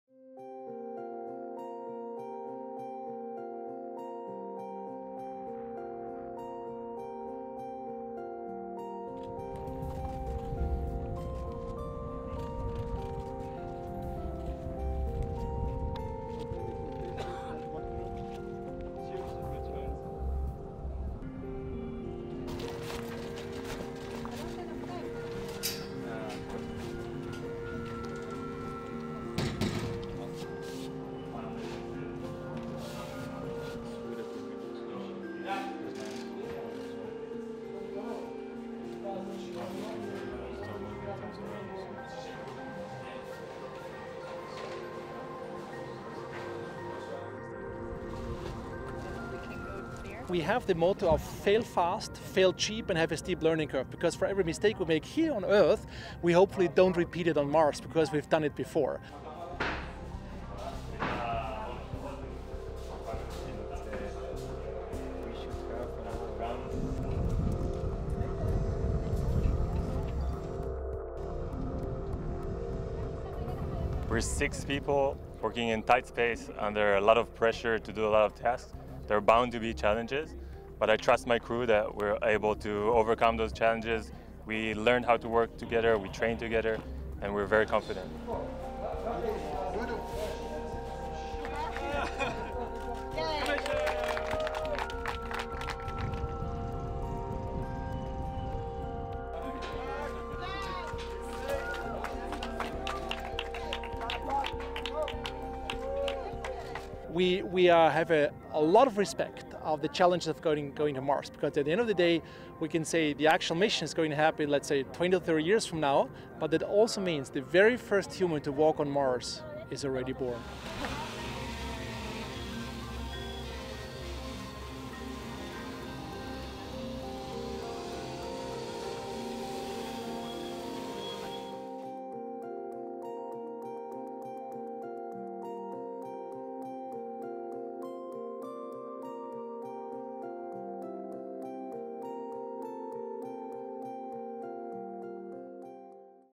Scientists simulate life on Mars in Israel’s Ramon Crater
In the Ramon Crater in southern Israel's red, hilly desert, a team of scientists have begun simulating what it will be like to live for about a month on Mars.

Team members sleep, eat and conduct experiments inside their AMADEE-20 habitat, while mock space suits fitted with cameras, microphones and self-contained breathing systems are worn outside.

The Austrian association is running the project together with the Israel Space Agency and local group D-MARS.

A number of recent Mars probes have captivated astronomy fans across the world with robotic rovers like NASA's Perseverance and, for the first time, the helicopter Ingenuity, offering a glance of the planet's surface. But a manned mission is likely more than a decade off.

With AMADEE-20, which was supposed to happen in 2020 but was postponed due to the COVID-19 pandemic, the team hopes to bring new insight that will help prepare for that mission, when it comes.